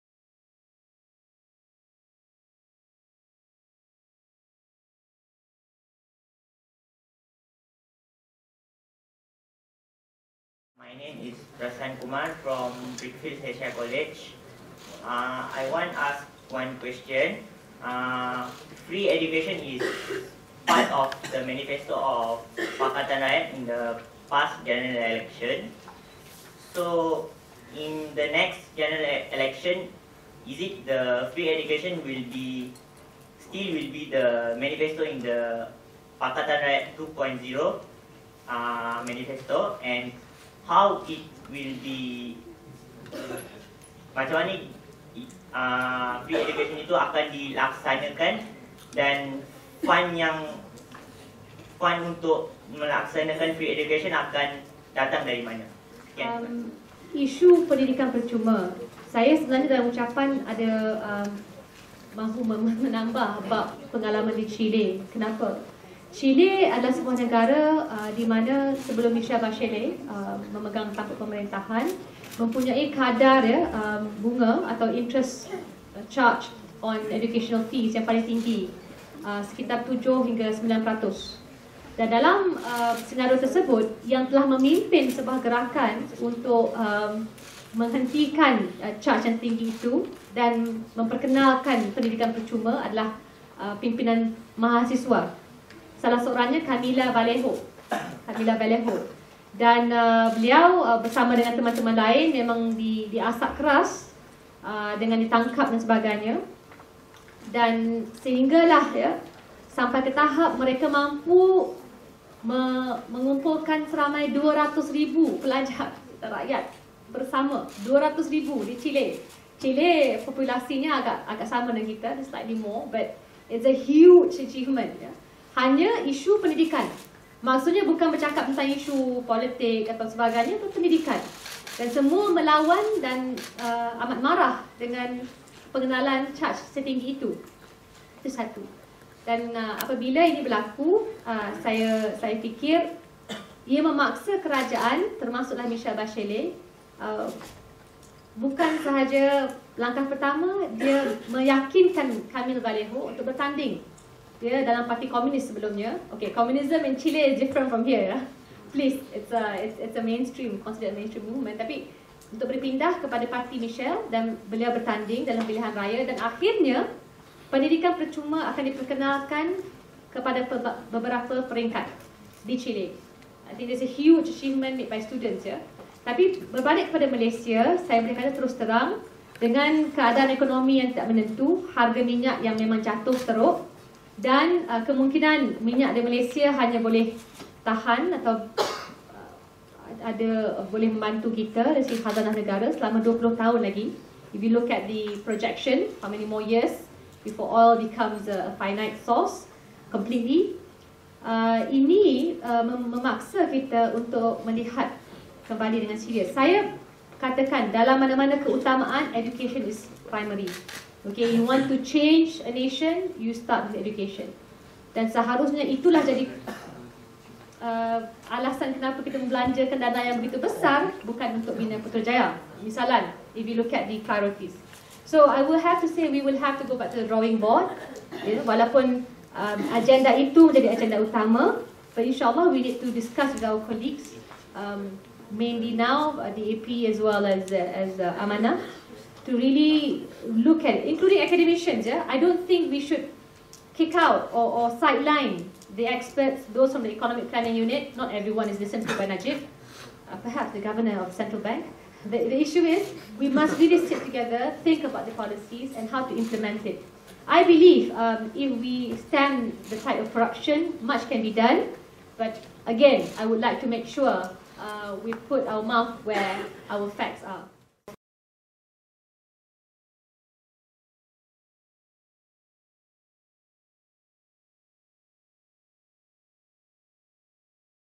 (Q&A) Nurul Izzah: You Want To Change A Nation, You Start With Education
Kongres Mahasiswa Kebangsaan, Ampang Jaya, Selangor 13/09/2015